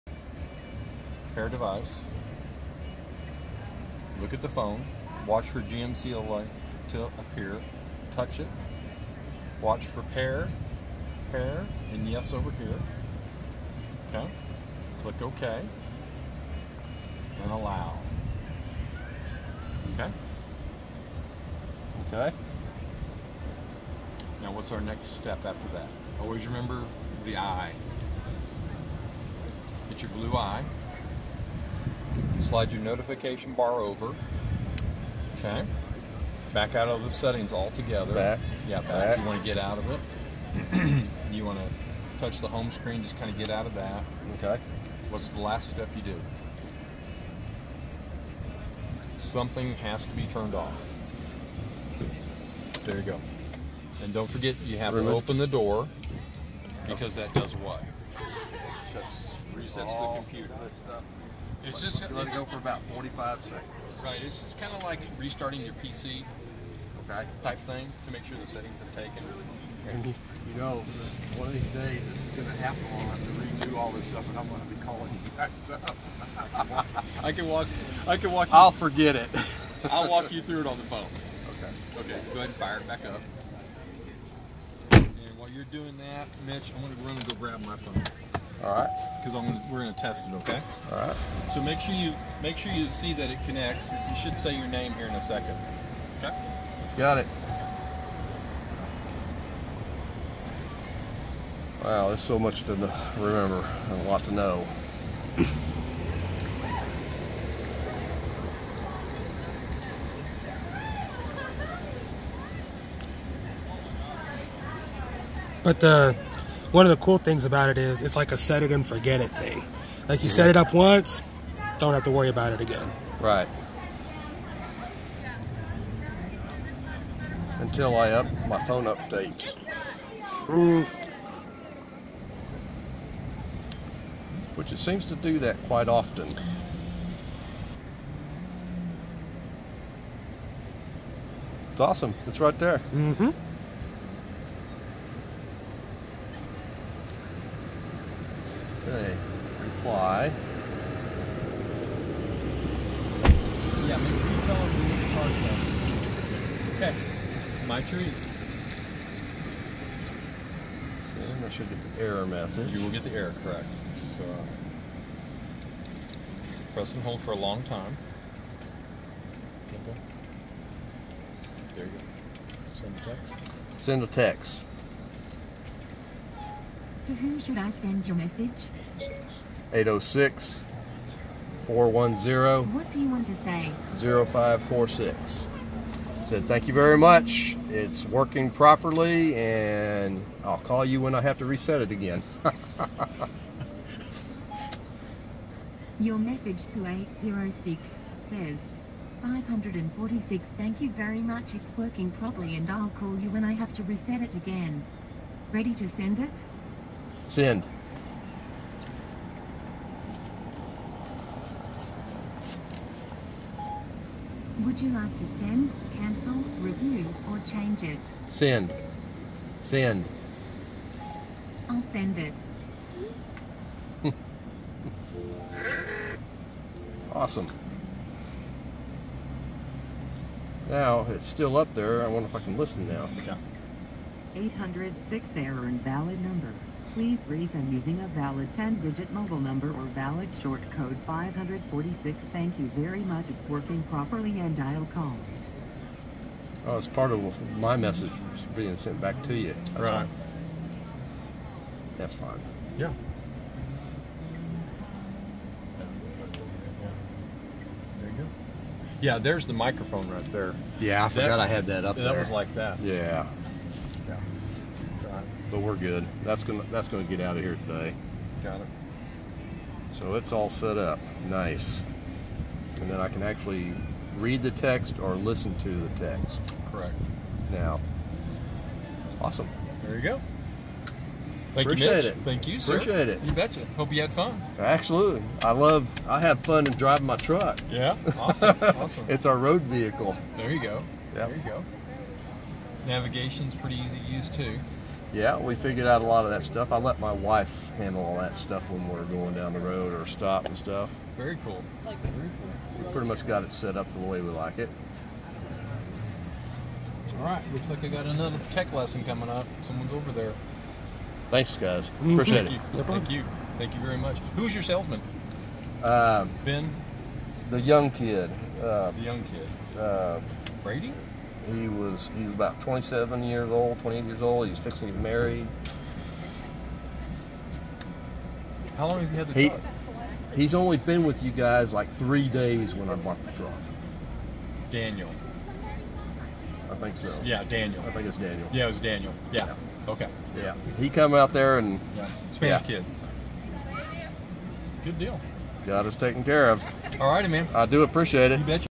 Buick GMC Tech Clinic Canyon Farmers Market
This is my Buick GMC Tech Clinic in 2018 summertime hot and dry. I will host these events on Saturday at the Canyon Community Farmers Market on Saturday mornings. Just a little of what I do, I really enjoy helping my clients know and understand what their vehicles need and are doing.